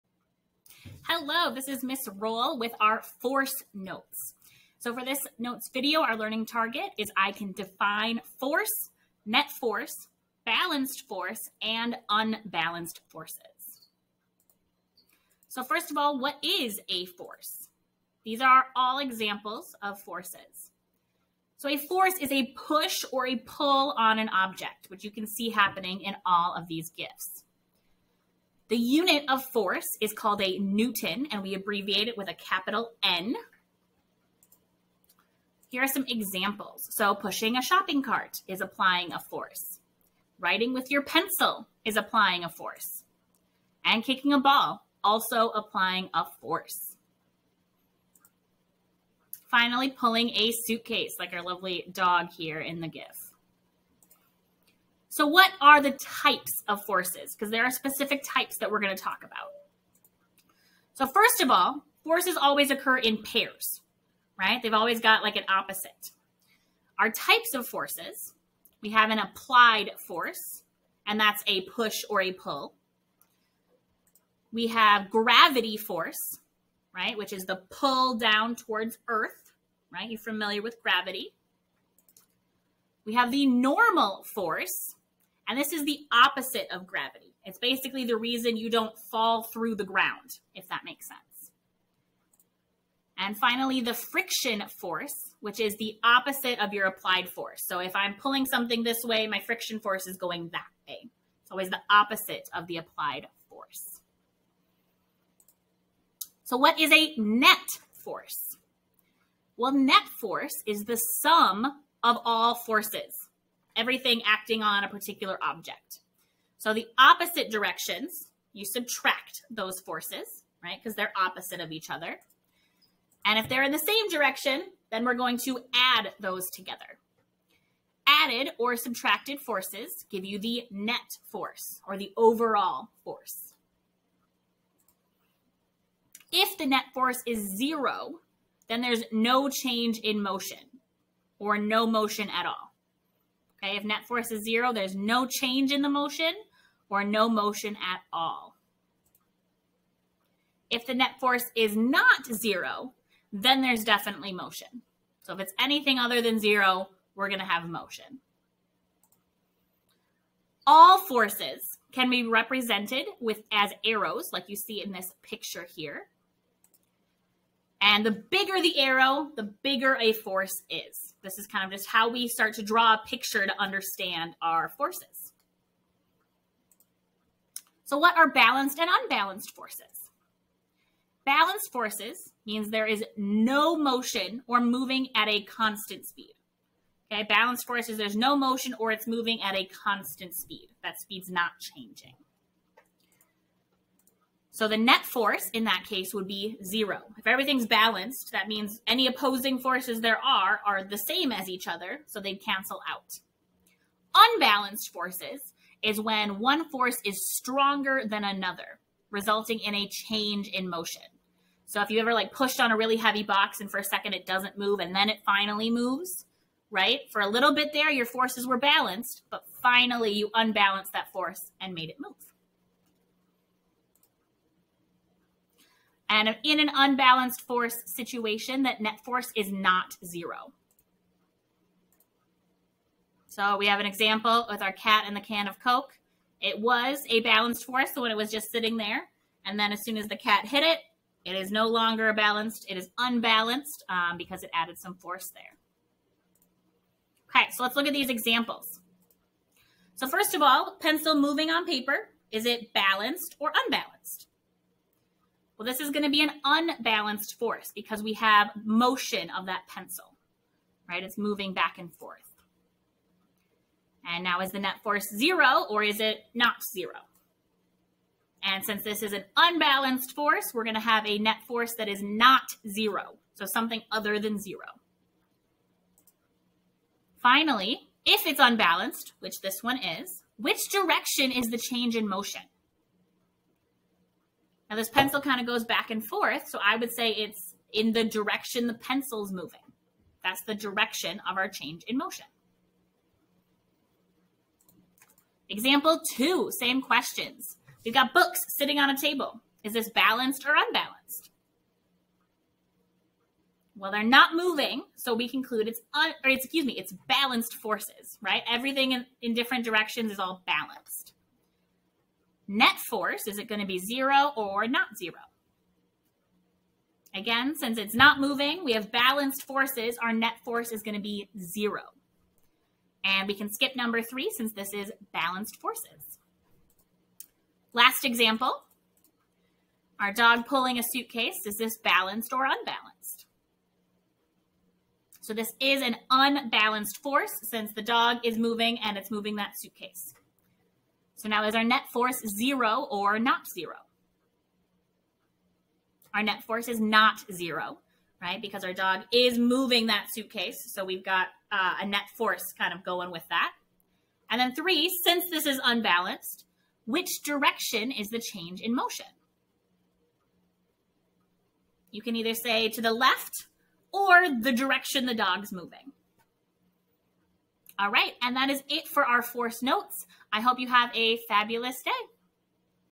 Force Notes
Notes on forces with a few quick examples.